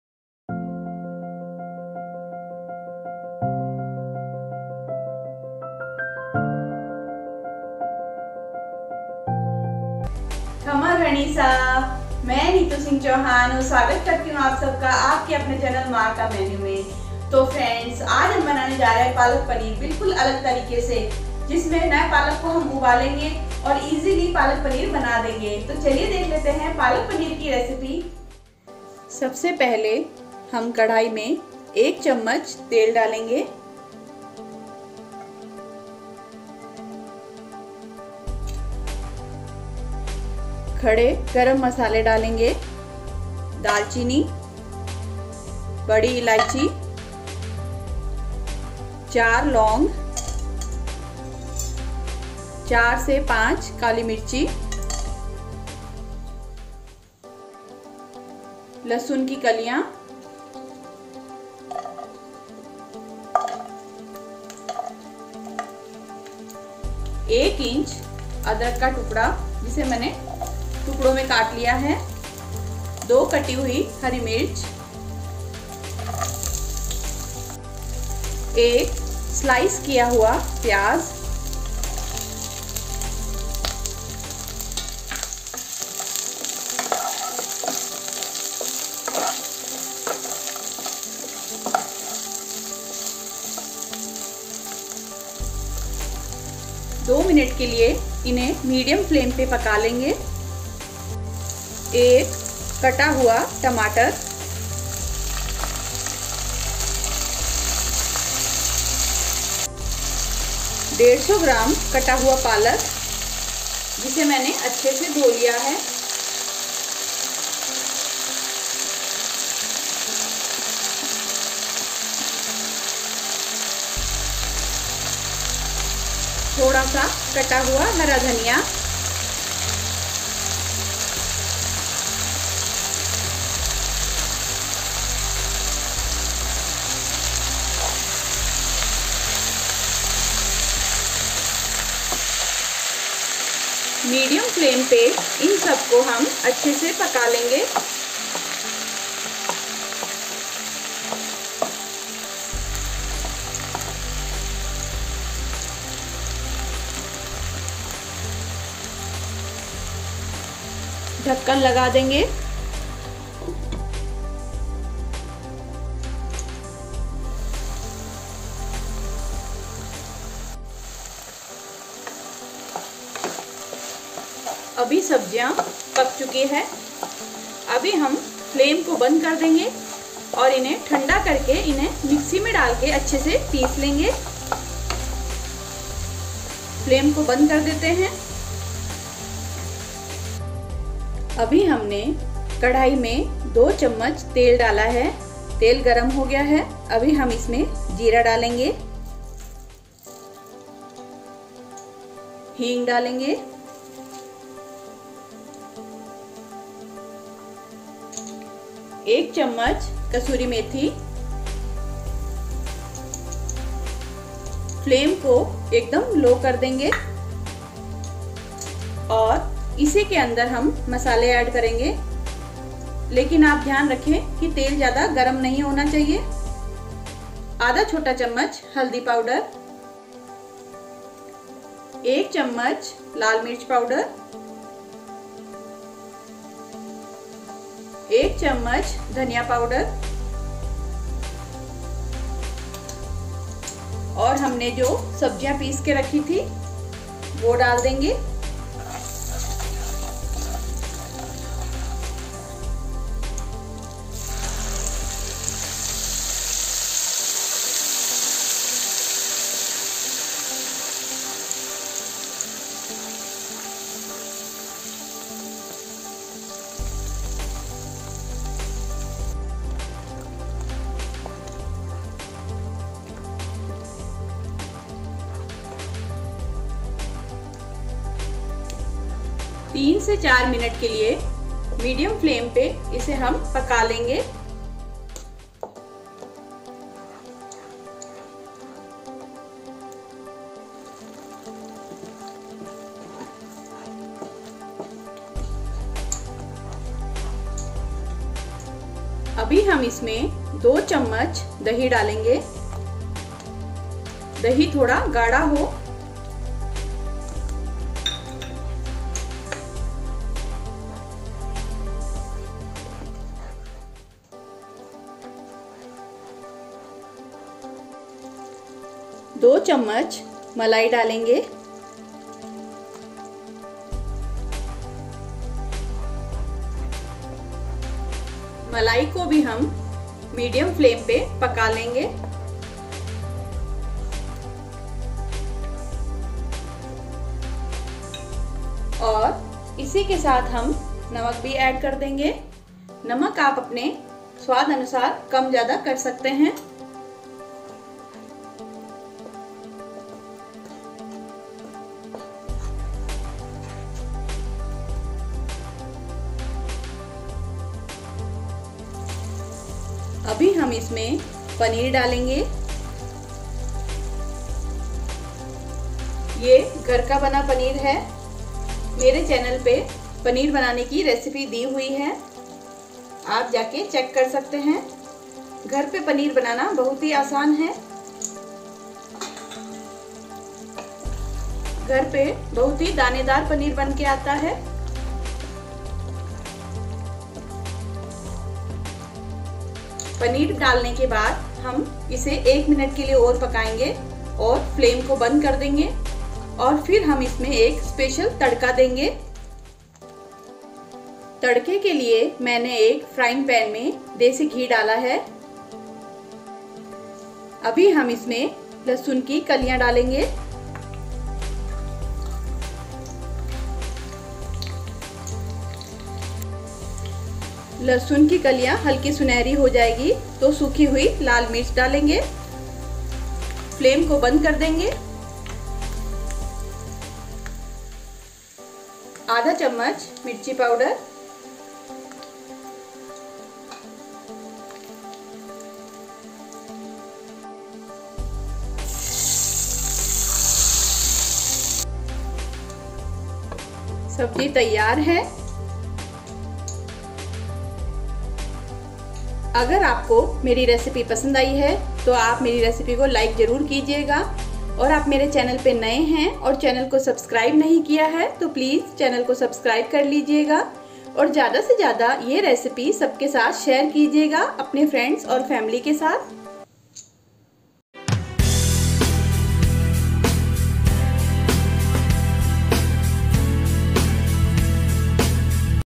बिना पालक को उबालें बनाये स्वादिष्ट पालक पनीर उंगलियां चाटते रह जाओगे | Maa Ka Menu
Palak Paneer | बिना पालक को बॉईल करे बनाये पालक पनीर | Maa Ka Menu

Palak Paneer is one of the most popular Indian dish made with succulent paneer cubes (Indian cottage cheese) in a smooth spinach sauce. vibrant green palak paneer recipe is made with fresh spinach leaves (palak in Hindi), aromatics, herbs and spices. Make this super delicious spinach paneer recipes with my easy step-by-step video.